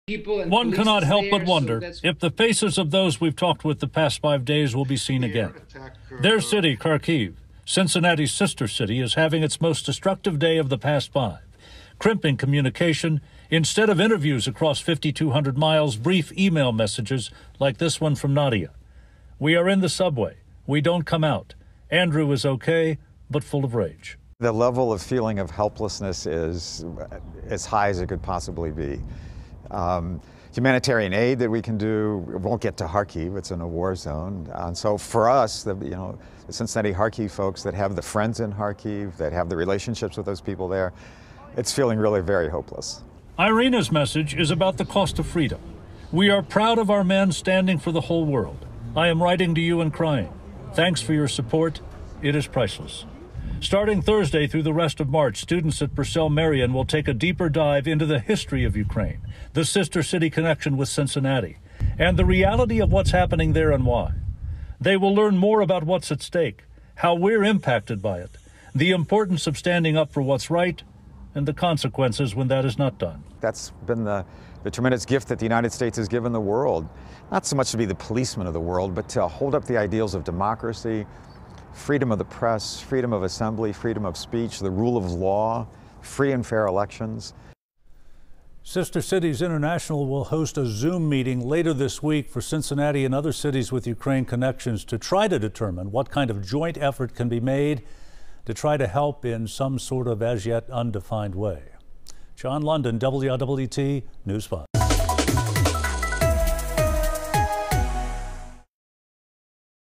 WLWT speaks with organizers of Cincinnati-Kharkiv Sister City Project
Their city, Kharkiv, Cincinnati's sister city, is having it's most destructive day of the past five. Crimping communication.